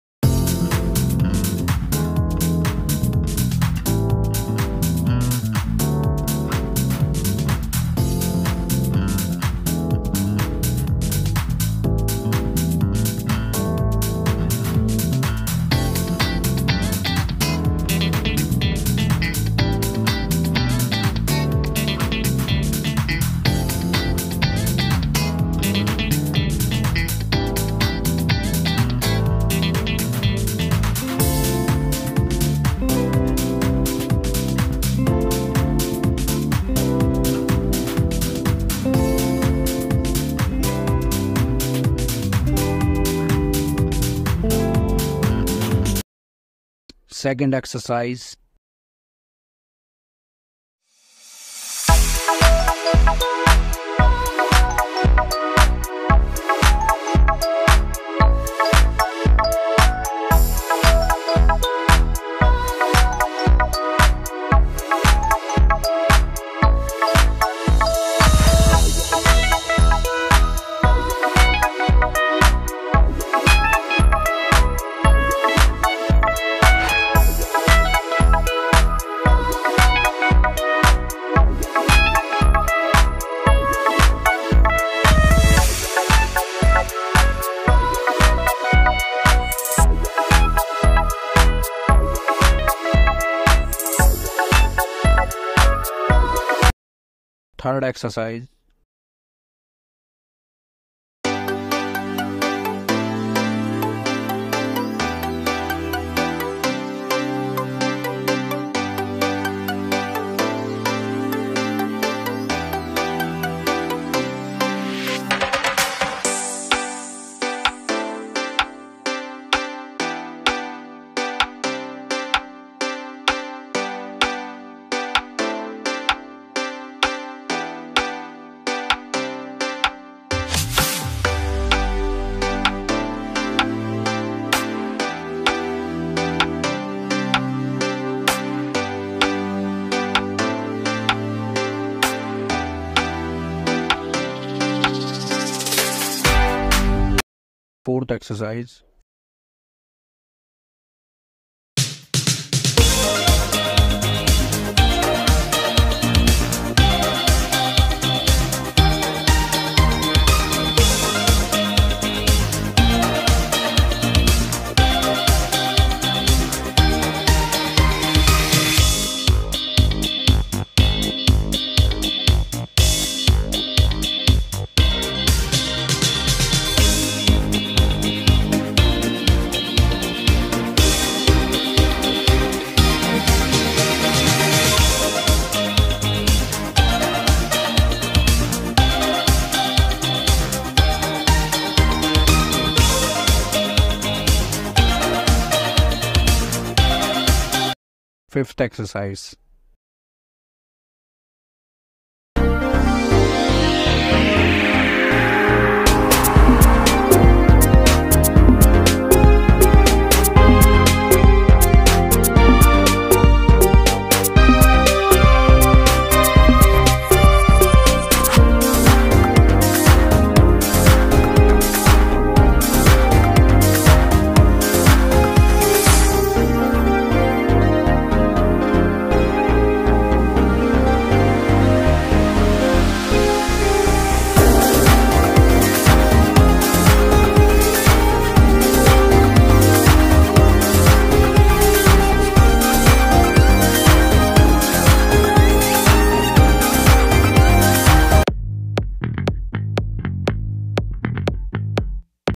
Best Abs Workout for Men
00:00 START
00:45 BRIDGE POSE SETU
01:35 CRAB TWIST TOE TOUCH
02:26 BUTTERFLY YOGA POSE
03:16 CARDIO LUNGES
04:07 ADDUCTOR STRETCHE
04:12 SUBSCRIBED AND THANKS FOR WATCHING